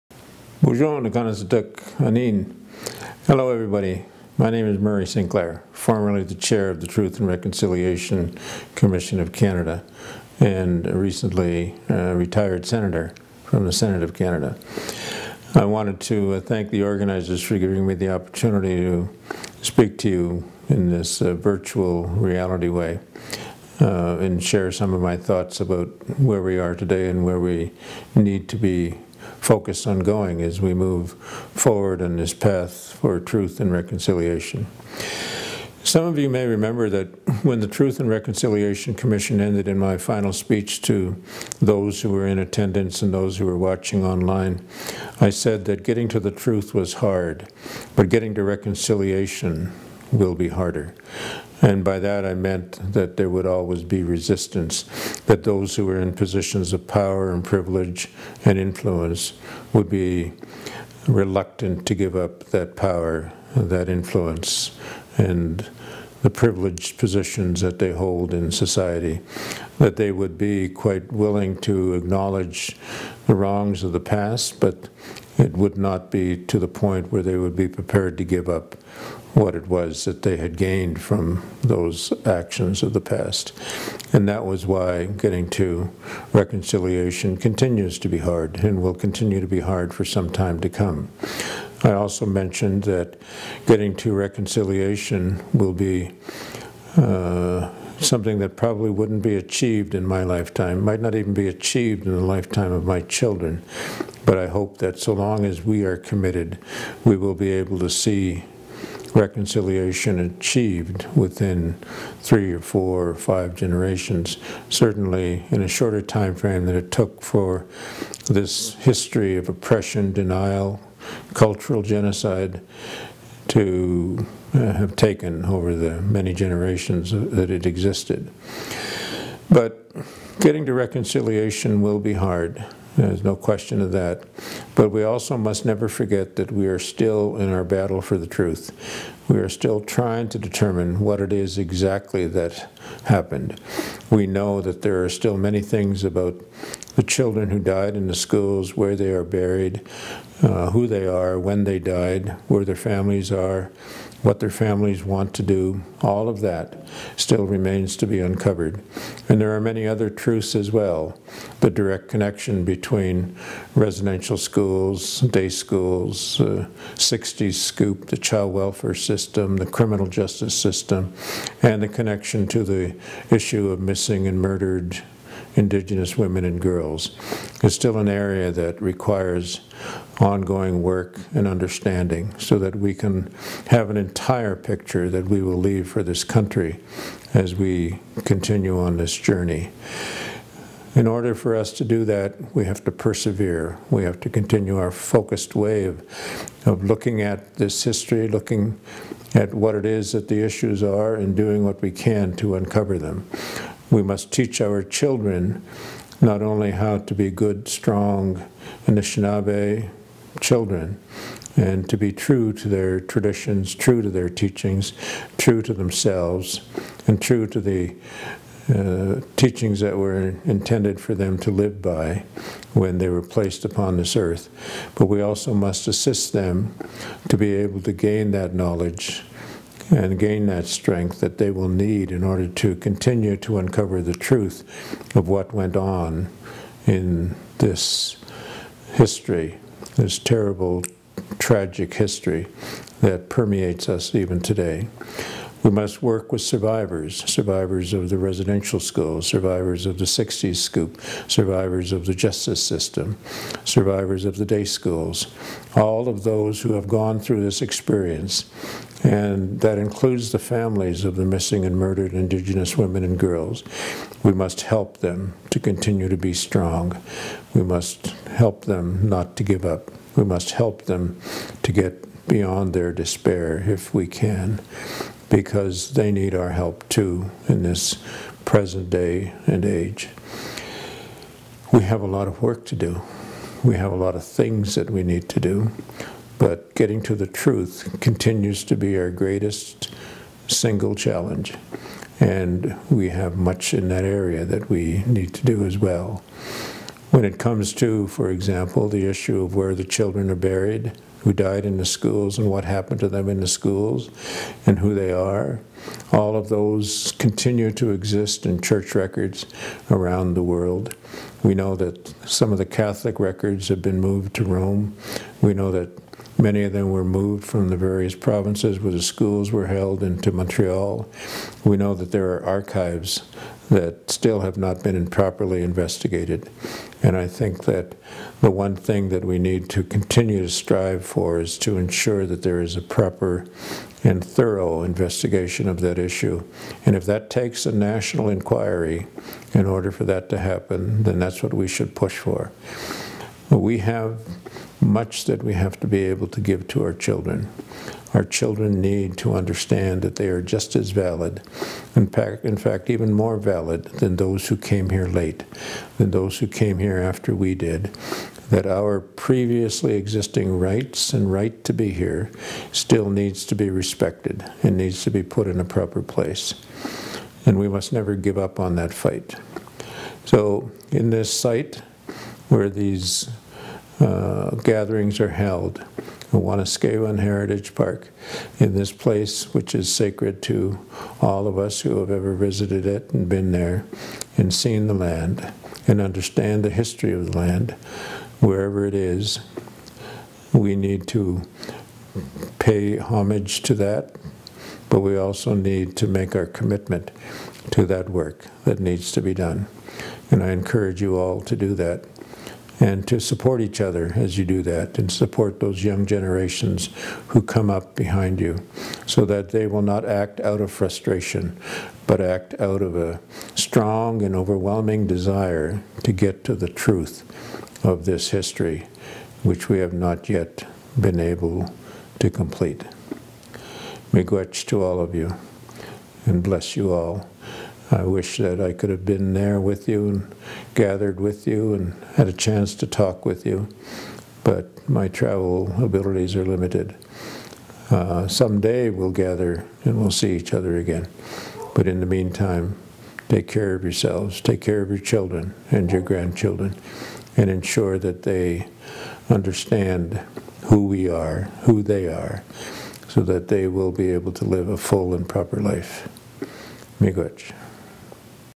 Honourable Murray Sinclair
The Honorable Justice Murray Sinclair offers his thoughts on Truth and Reconciliation Day at Wanuskewin.